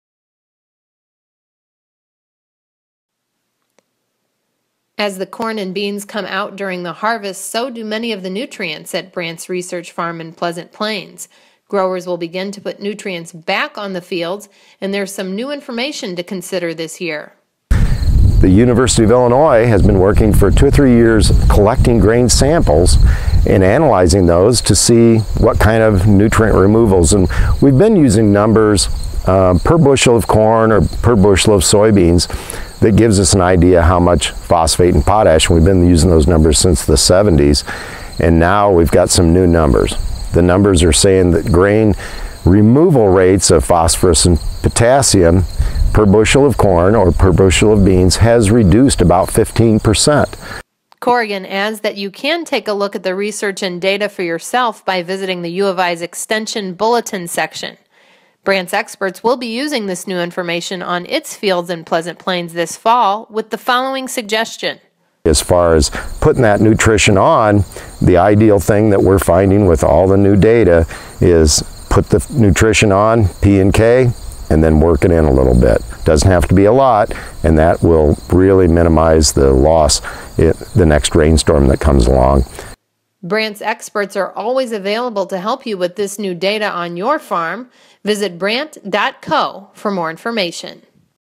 BRANDT Agronomy Update: Nutrients Removed After Harvest
The University of Illinois has been collecting data over the past few years collecting grain samples to see exactly what nutrients are being removed. Using this new data can give growers a better idea of what nutrients and how much are needed in fields.